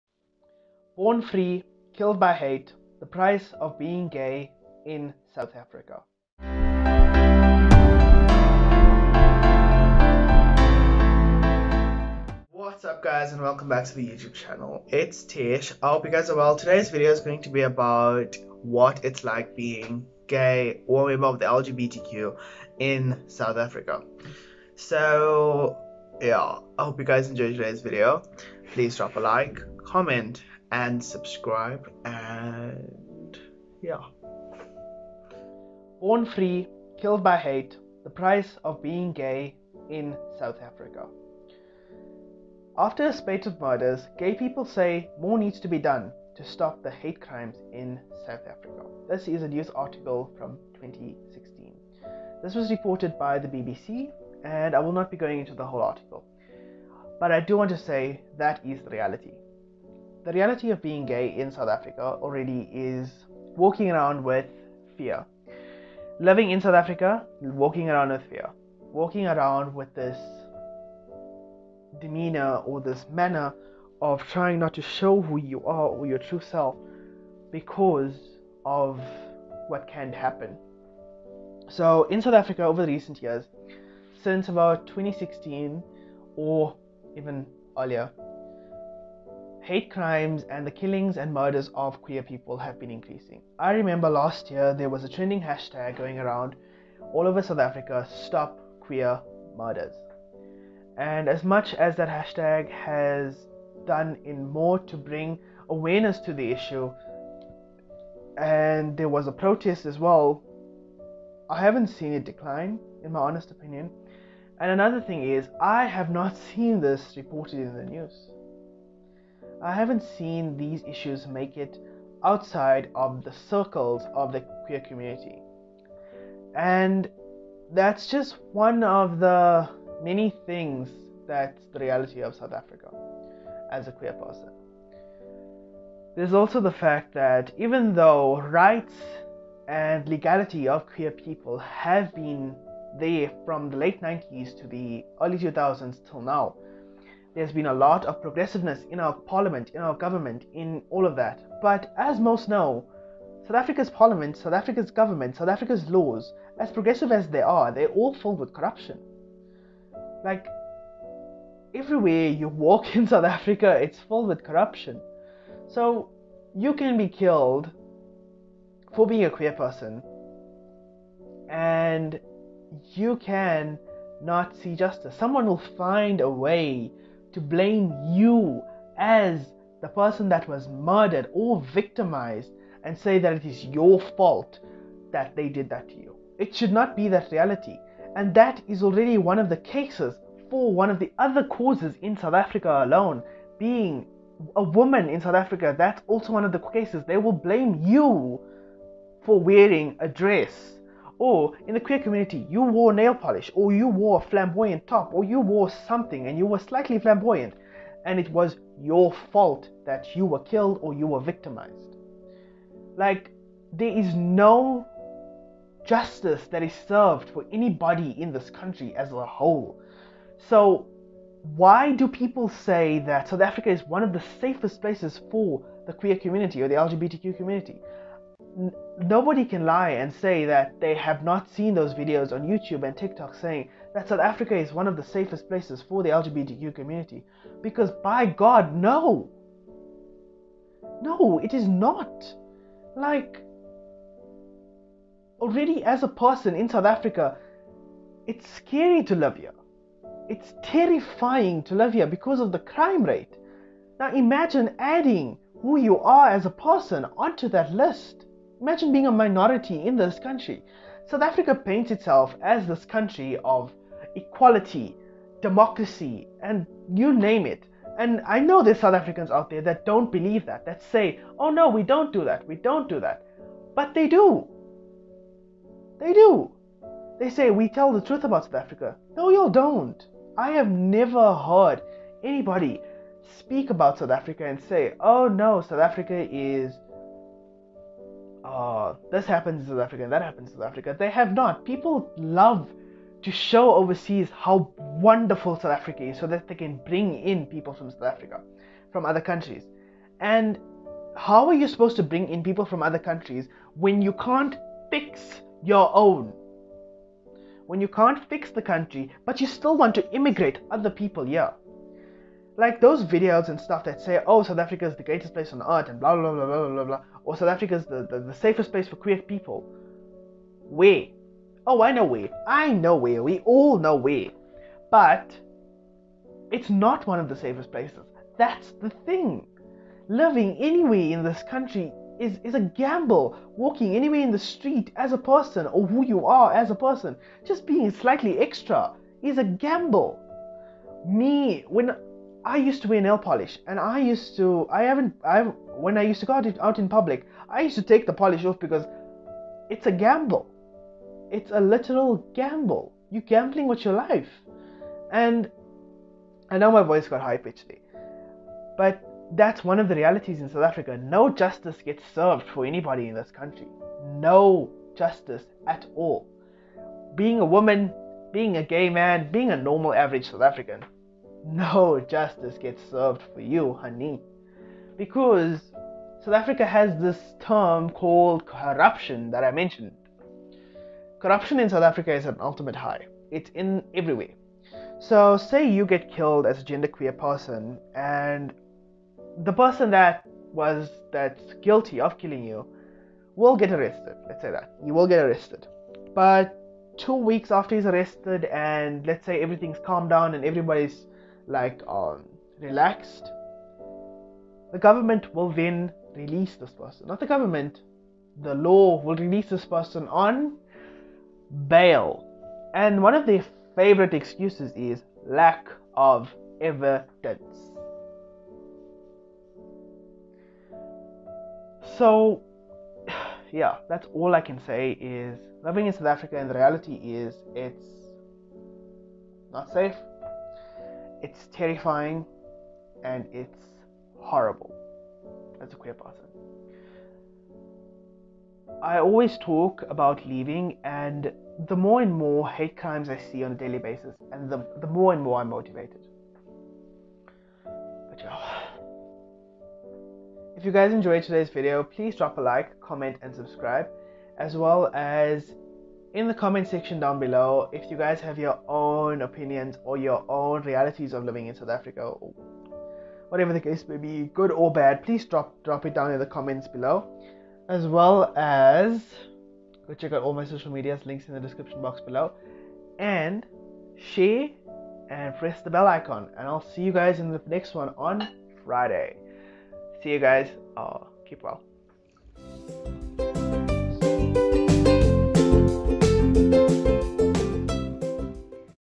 Being Gay in South Africa The Truth About It!
Howsit guys todays video is about some of the harsh realities of living in South Africa as a Queer person amd some of the harsh realities of it

Previous Video:
Why are South Africans leaving?

I have made some changes to my channel:
Mondays - Random Videos(Vlogs, etc)
Wednesday- Personal videos(Vlogs, etc)
Friday- History Videos

Hastags